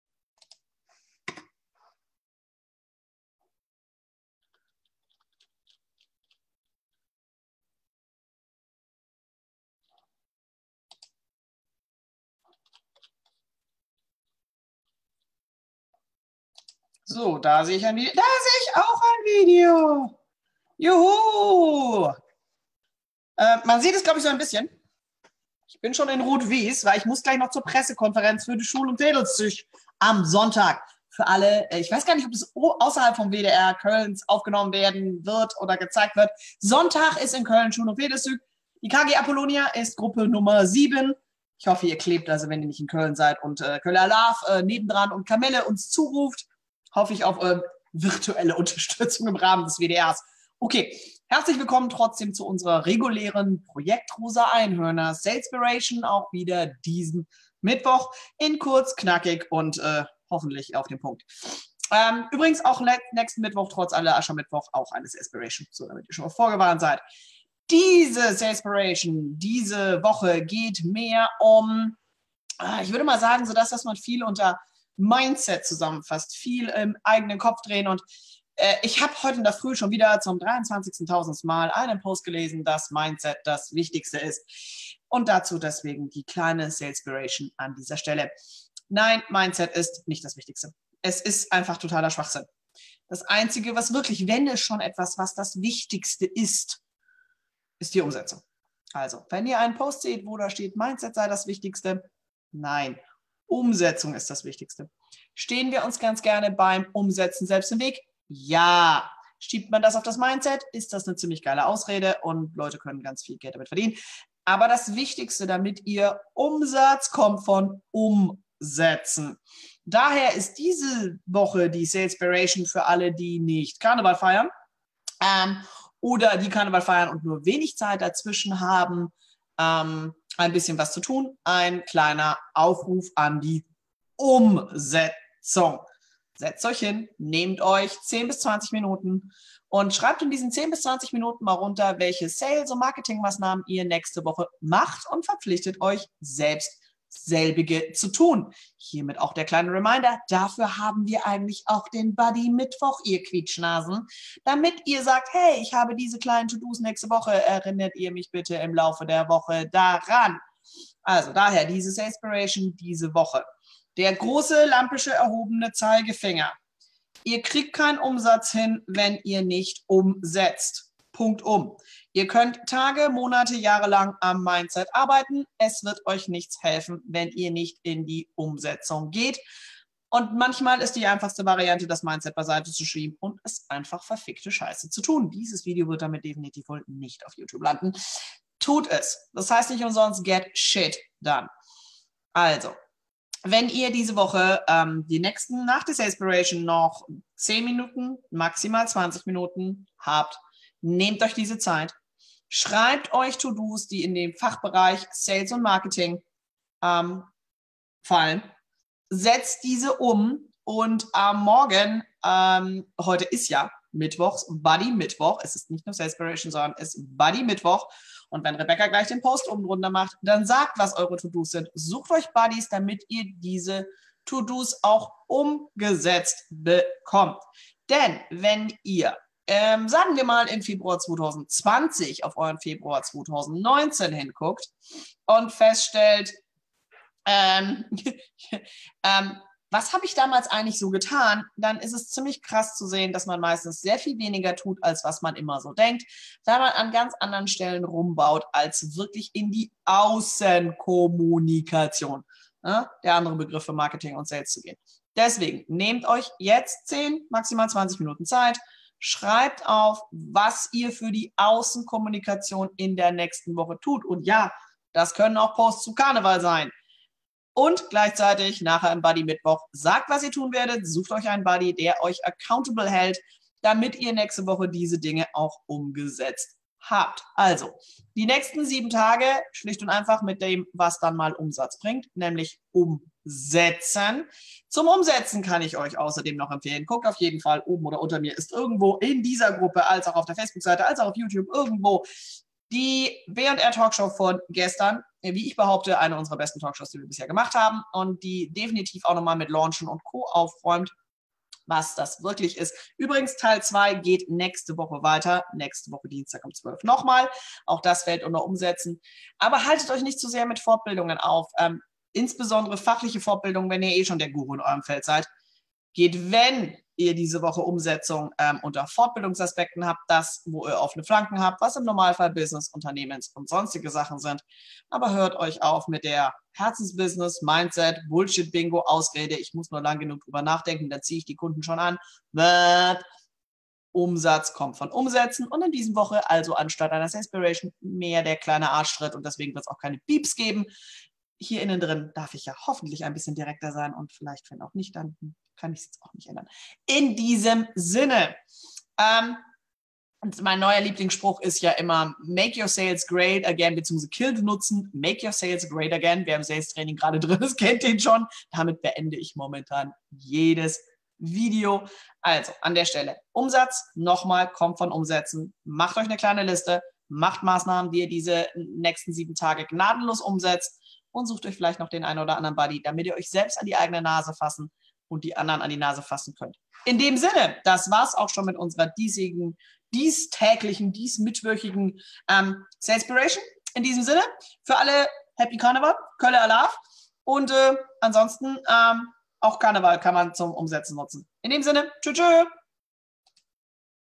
Salespiration - Umsetzen statt Mindset
Umsatz kommt von umsetzen!
Warum es nicht reicht sich auf sein Mindset zu konzentrieren.

Jeden Mittwoch gibt es ein Live-Video zum Thema Sales in der Einhörnergruppe, hier ist der